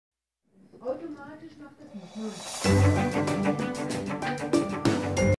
DJ Malenka & Ekambi
Ich lege auf:
Internationale DJ Sounds, House Basic bis Jazz...

 Booking= CMT Management Paris, bin zur Zeit viel in München und auch spotan einsetzbar,
würde gerne einmal ein Gespräch haben und die Technik inspirieren, sowie einen Rundgang am Tag machen.

Lg Eure Malenka